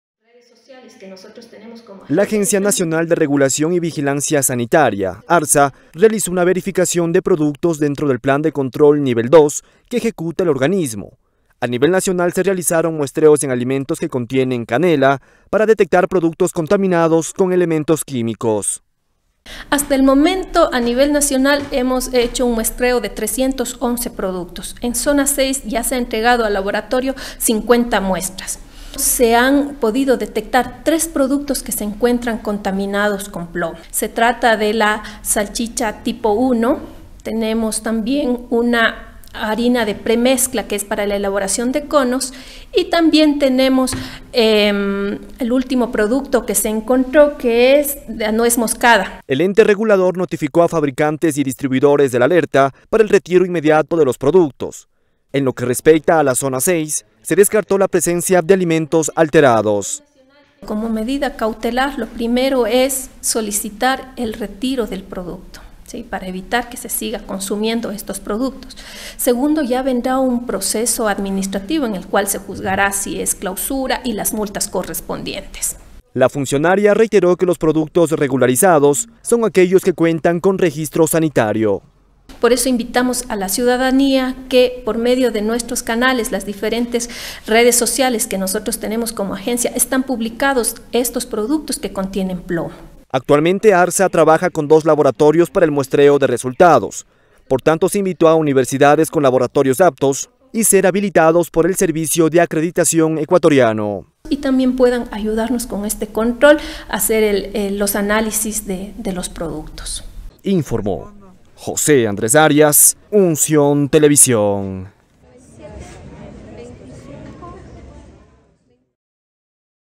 ARCSA no ha detectado productos contaminados con plomo en la Zona 6
La Agencia Nacional de Regulación, Control y Vigilancia Sanitaria de la Zona 6, descartó el expendio de alimentos contaminados con plomo en Azuay, Cañar y Morona Santiago.